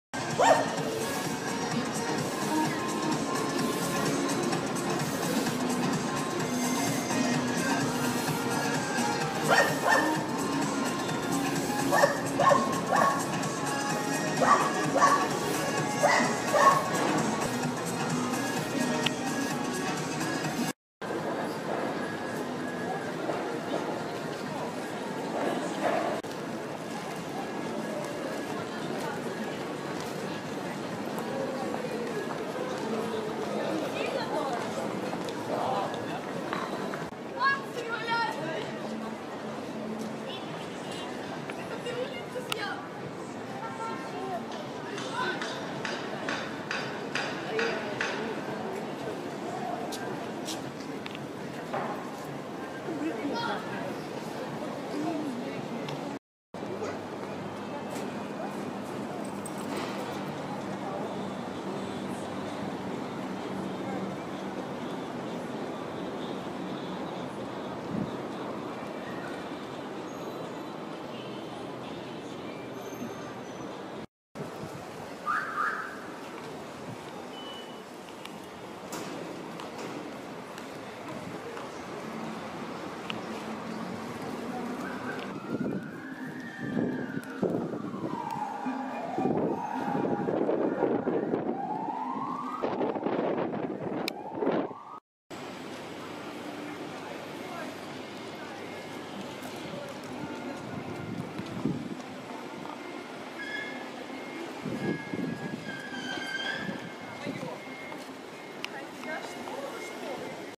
Почему позволяют творить уродство в центре Одессы?
Одесса, осень 2015.